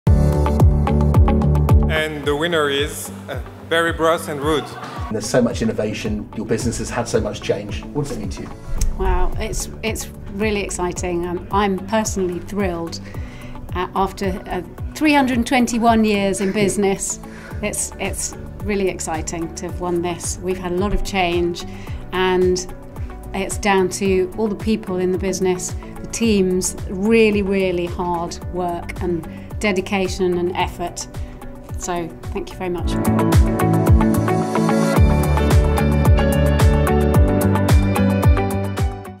IWC 2019 Large Independent of the year
Berry Bros. & Rudd has many strings to its bow. The London shop has really come into its own, reflecting its history but with a contemporary feel. They aren’t standing still and are stepping out of their comfort zone, by doing pop-ups and the Wine Car Boot. They have a sensational range of wines, which is incredibly well chosen, and a brilliant website.